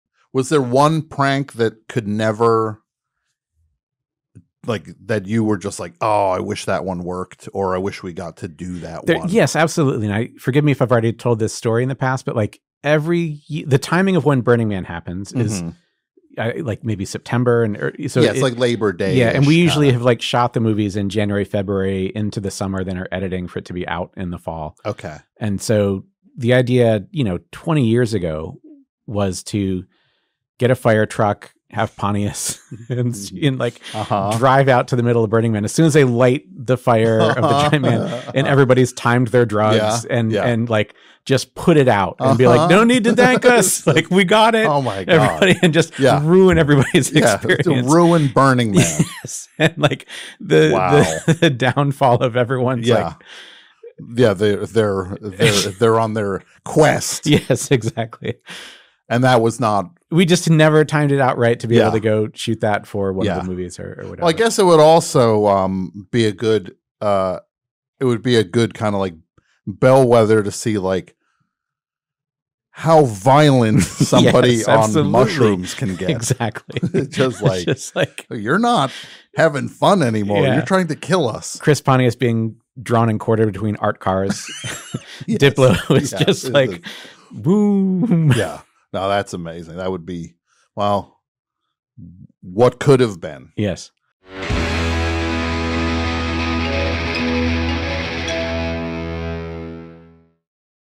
The Greatest Jackass Prank That Never Happened
Lance Bangs tells Tom about the Jackass plan to ruin Burning Man and why it never happened.